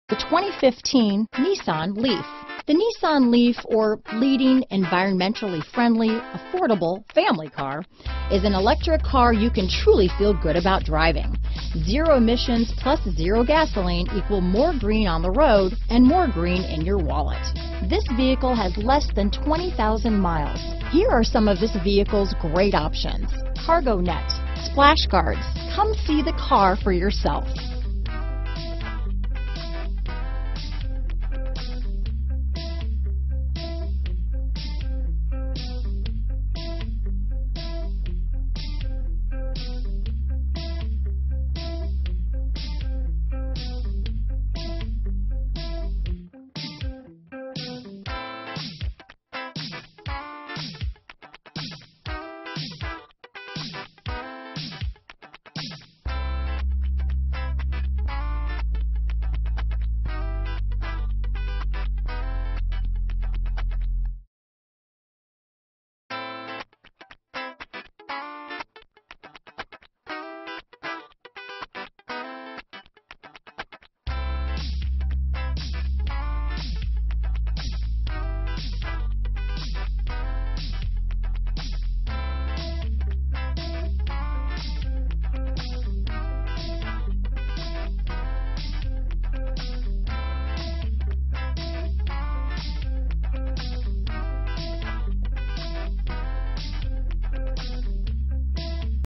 2015 Nissan LEAF Vestal, Binghamton, Sayre, Ithaca, Horseheads, NY 62A
Morningsky Blue Used 2015 Nissan LEAF available in Vestal, new York at Serafini Nissan Volvo. Servicing the Binghamton, Sayre, Ithaca, Horseheads, NY area.

Serafini Nissan Volvo
3101 Vestal Pkwy E

6.6 kW Onboard Charger,Cargo Net,Charge Package,Floor Mats & Cargo Area Mat,Quick Charge Port,Splash Guards